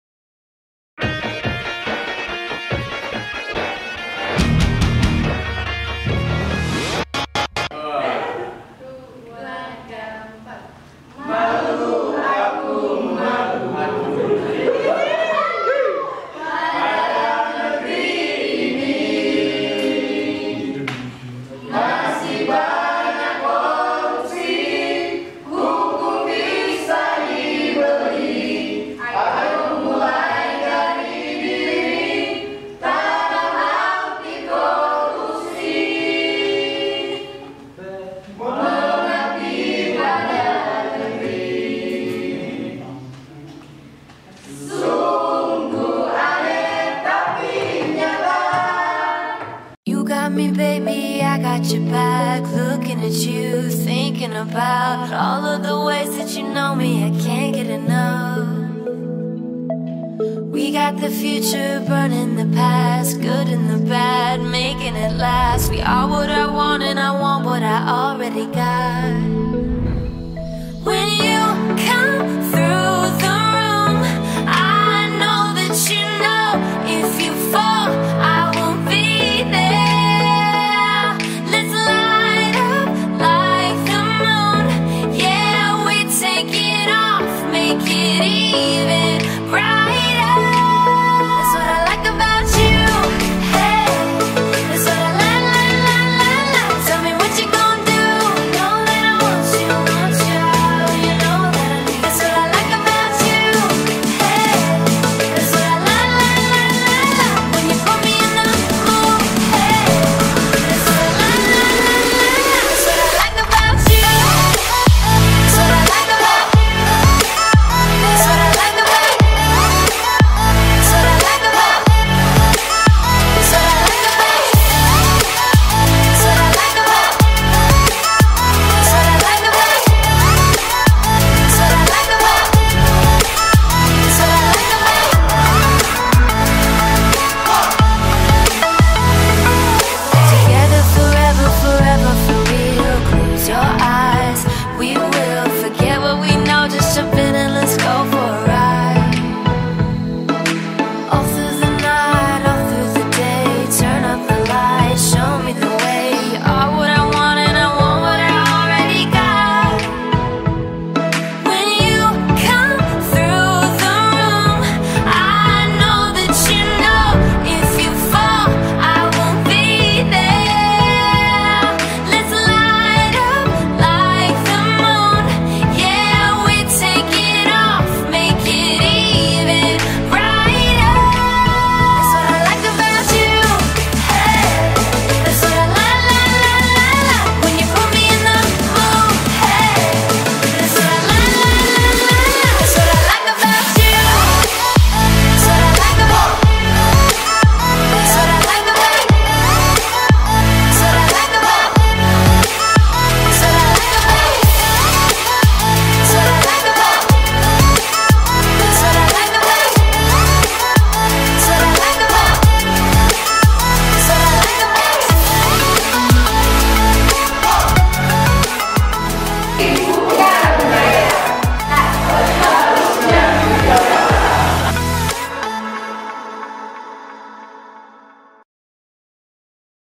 Antikorupsi Angkatan 26
Latsar CPNS angkatan 26 menerima materi anti korupsi di komplek mega dengan antusias. Tujuan di berikan nya materi anti korupsi ini agar kelak peserta menjadi PNS maupun pemimpin yang jujur dan berintegritas tinggi.